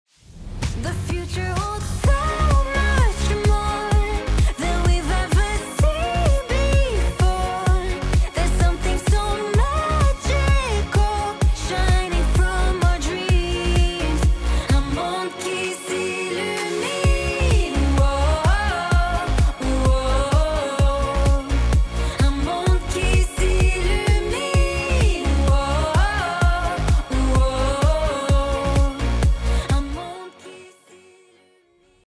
ShotonOPPO | Un viaje a Disneyland con el OPPO Find X5 Pro
¡Viajamos a Disneyland París para celebrar su 30º aniversario!

Acompáñanos para vivir esta celebración única en la vida, mientras capturamos el encanto de su magia nocturna que nunca se acaba.